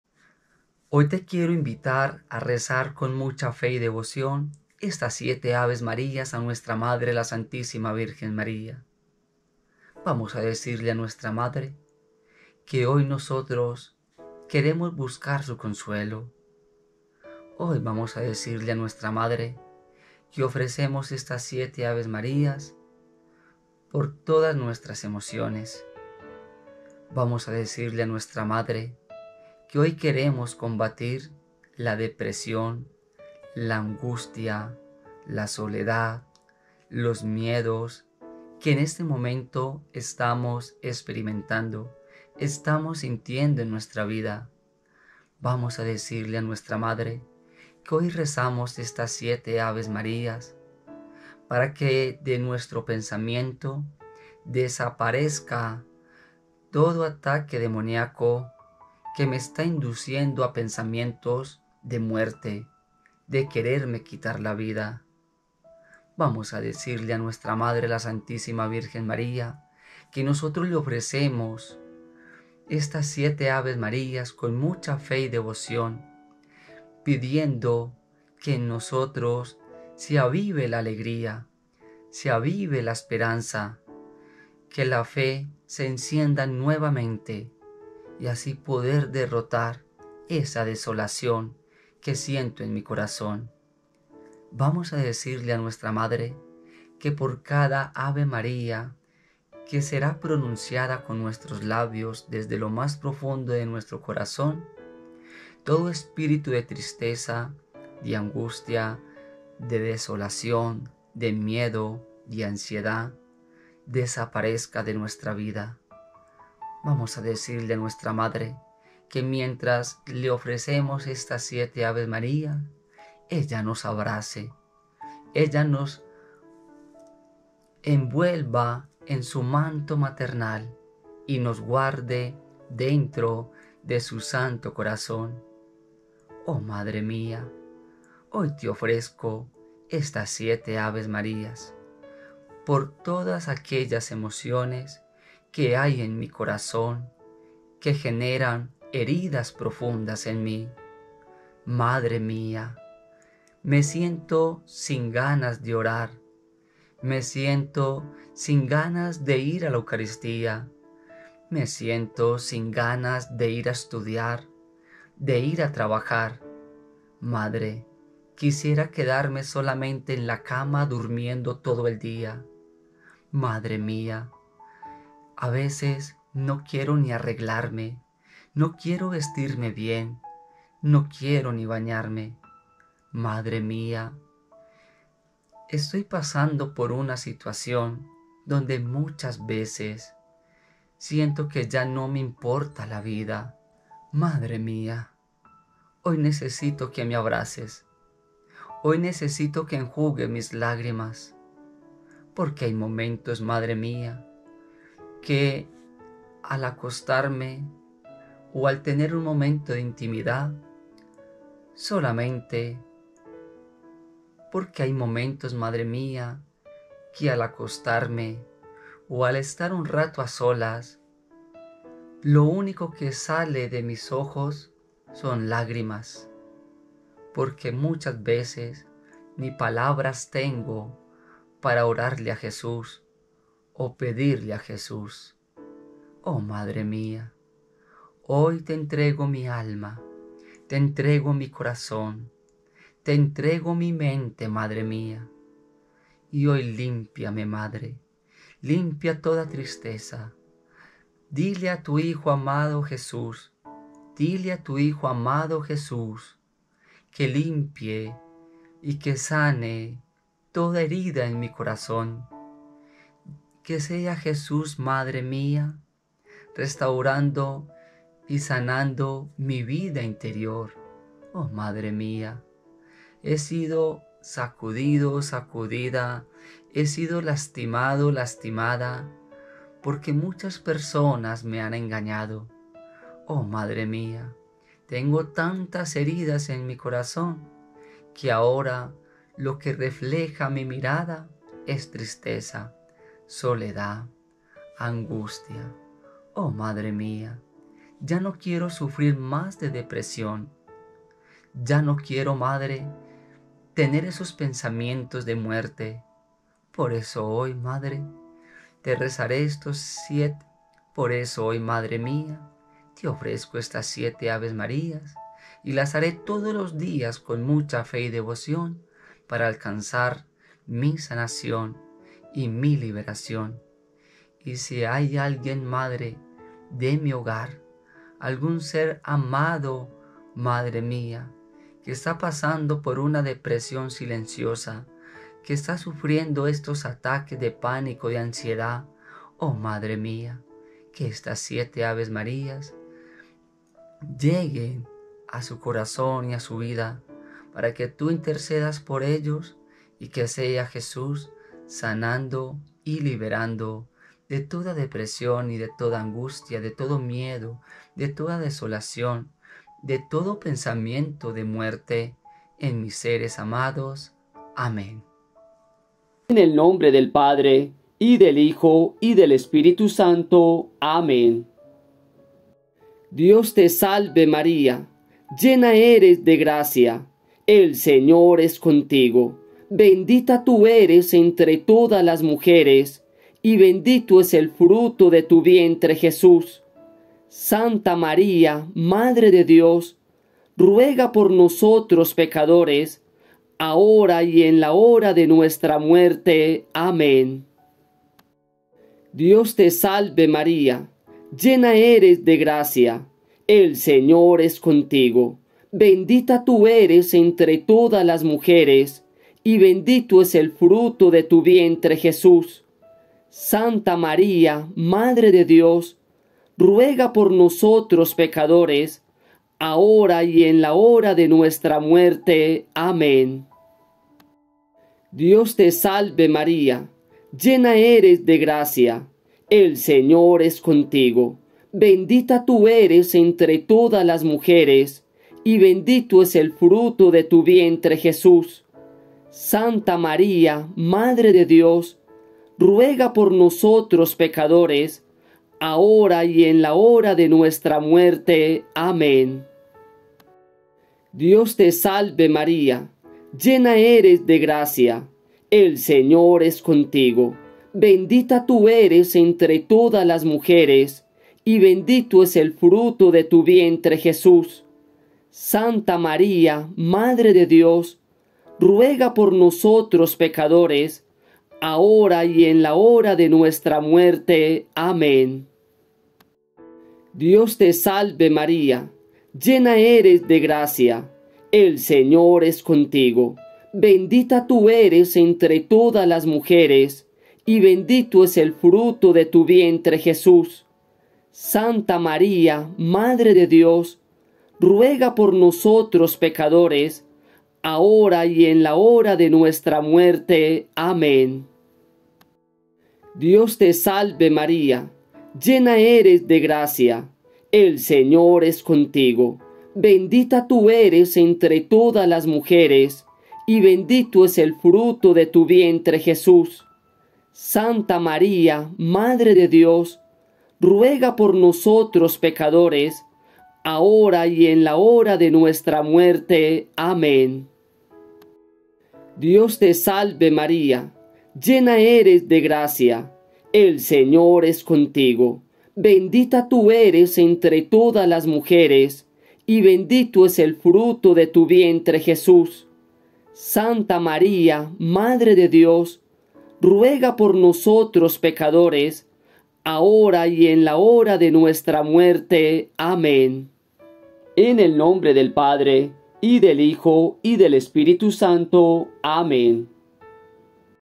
RECEMOS LAS SIETE AVES MARÍAS HOY JUEVES 12 DE SEP 2024. POR LA DEPRESIÓN
GRUPO de oración María SANTIFICADORA